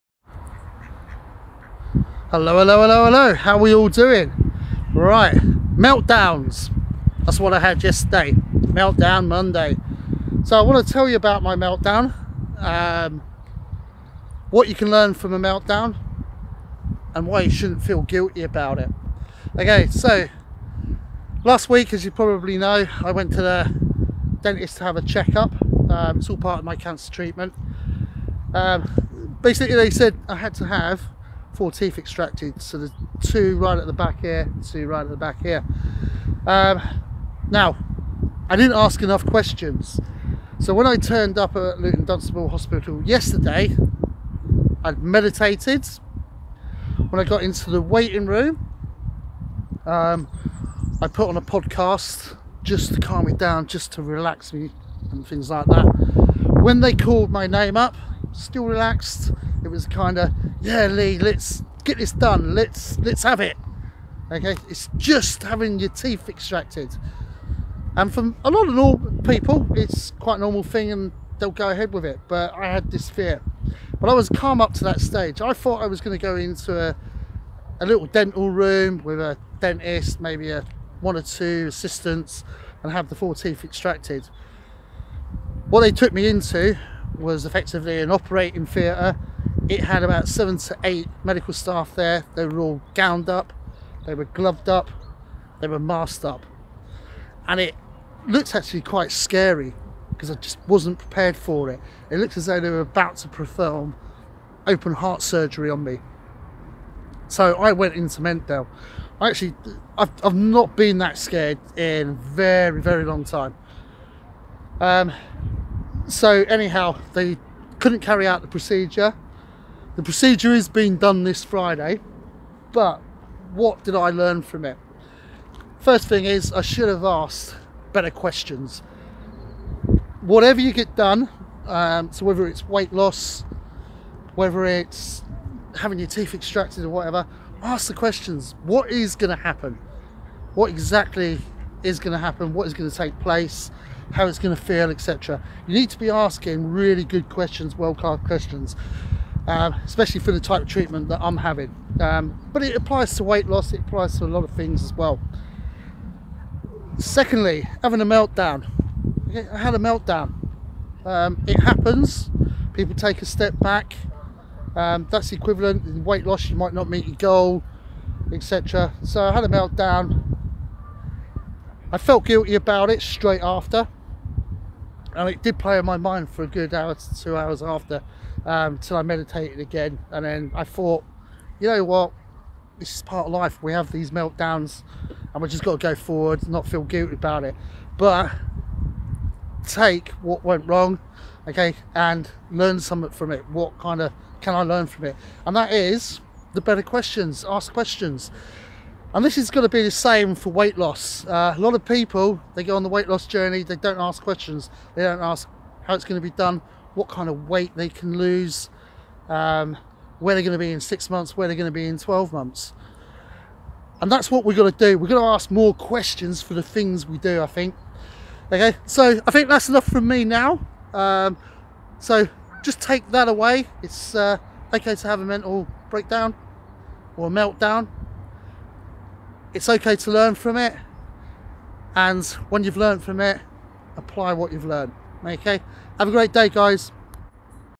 Monday Meltdown
Yesterday was 'Meltdown Monday' and it's ok to have a meltdown. Learn from it! I need to be asking more questions so I'm better prepared, so I don't have meltdowns in the future.